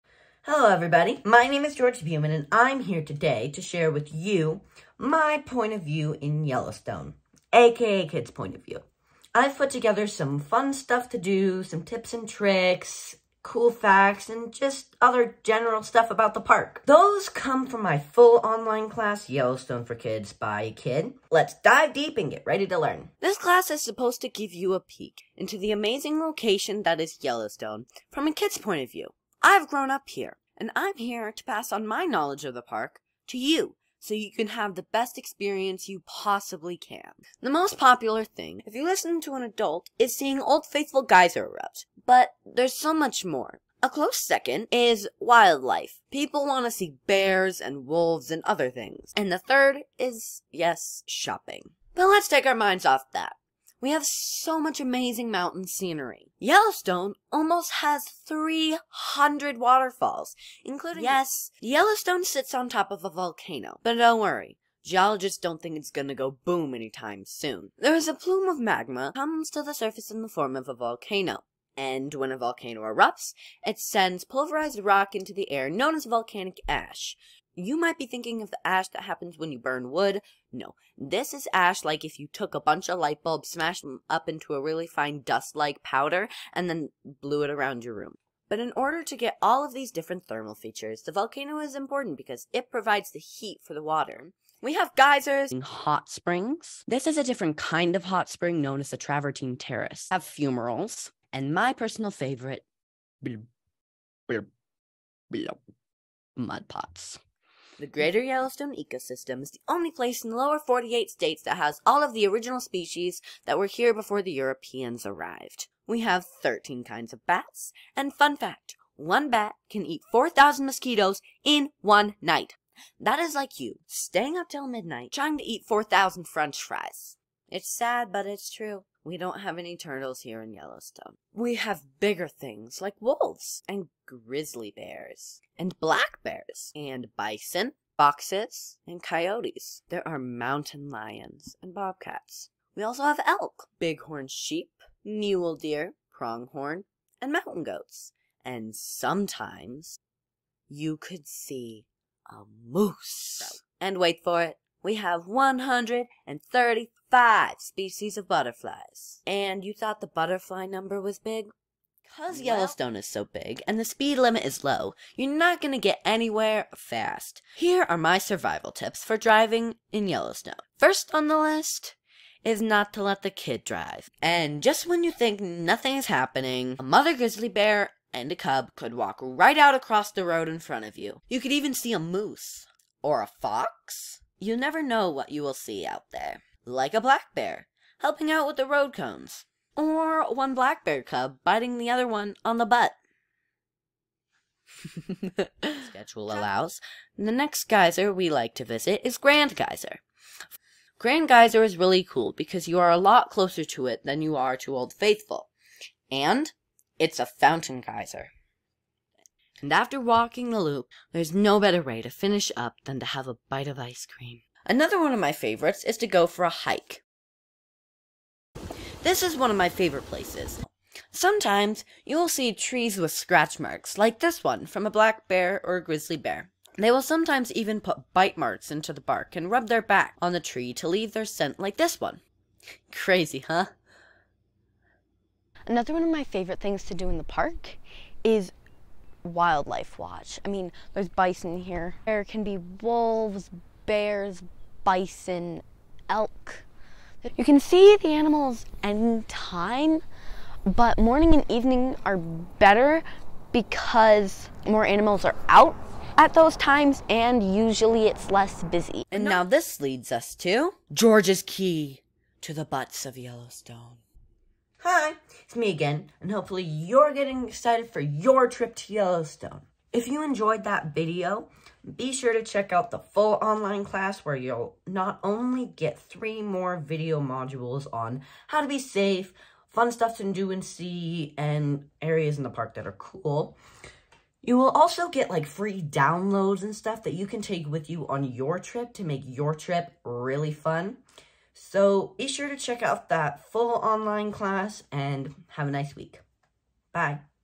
Yellowstone For Kids Introduction
Here's an intro to Yellowstone for Kids, by a Kid!, the ultimate kid-friendly guide to visiting Yellowstone with your family...from a kid's point of view.

In this super fun and informative online course I'll help you uncover the park's many secrets by providing a uniquely "kid" perspective on what makes Yellowstone amazing, and how to make your trip to Yellowstone an unforgettable experience for both parents and kids.